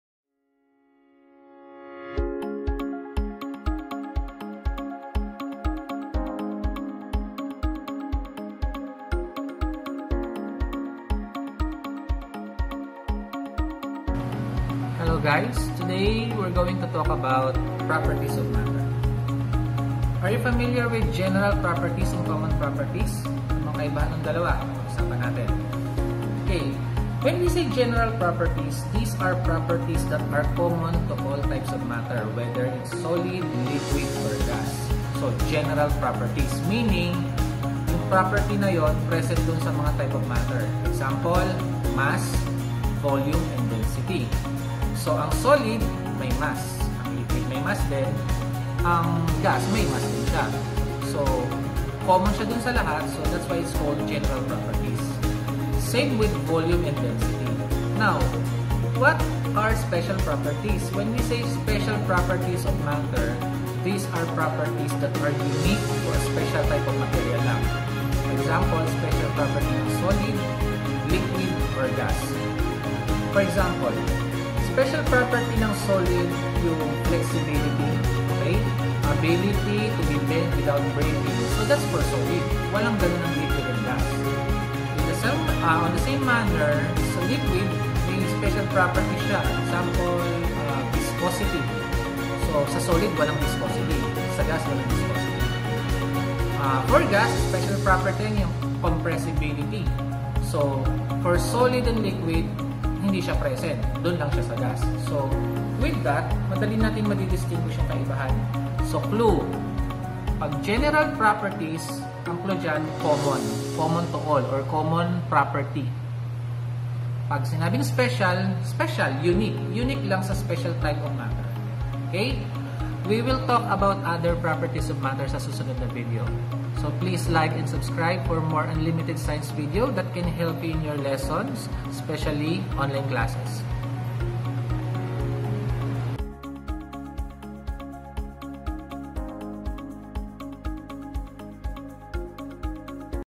GENERAL AND SPECIAL PROPERTIES OF MATTER
What is the difference between General Properties and Special Properties of Matter?